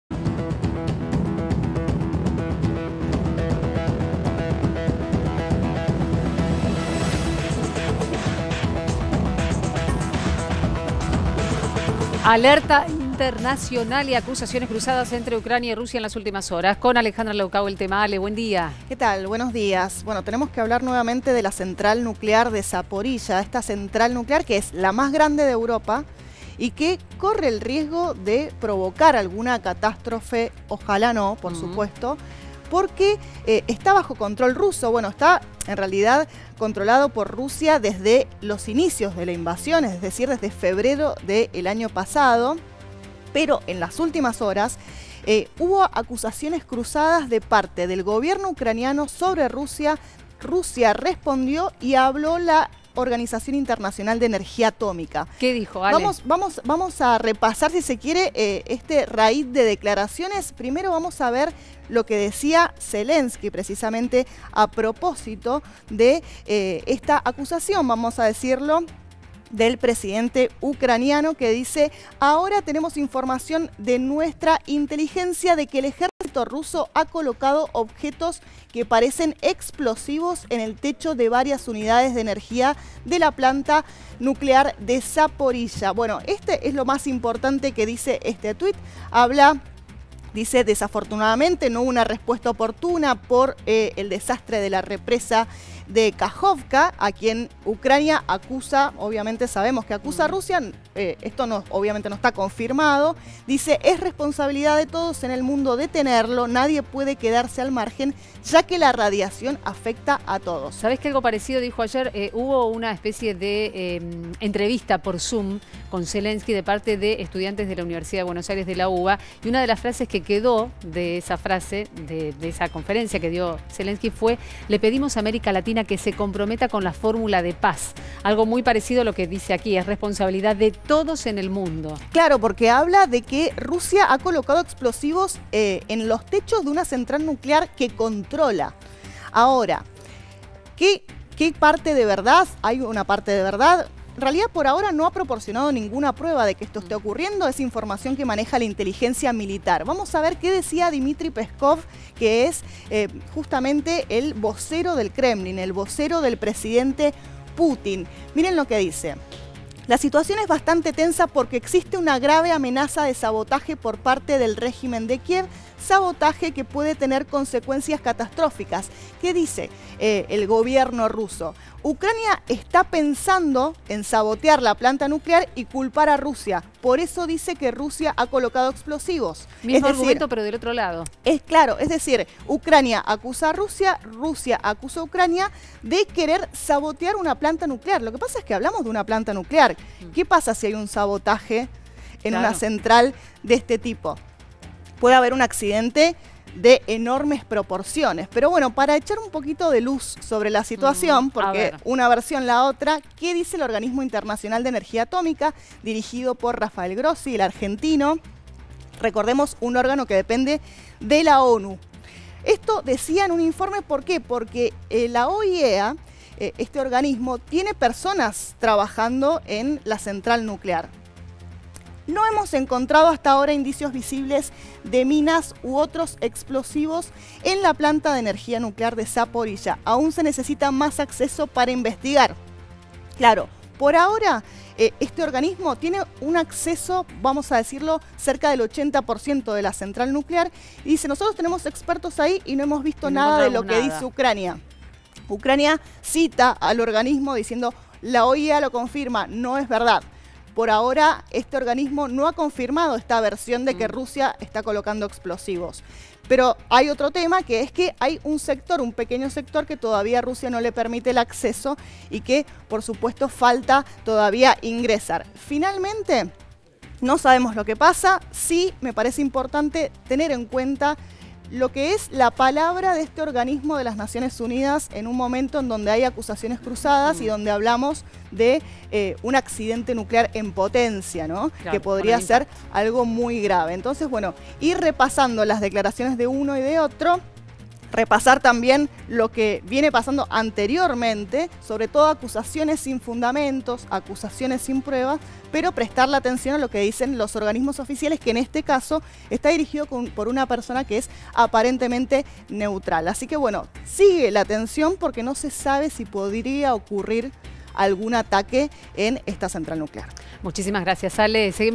Ucrania insiste en que Rusia prepara "un atentado terrorista" en la central nuclear de Zaporiyia
El organismo de control nuclear de Naciones Unidas exigió mayor acceso a la central nuclear de la sureña ciudad ucraniana, ocupada por tropas rusas, para "confirmar la ausencia de minas o explosivos" como afirman en Kiev.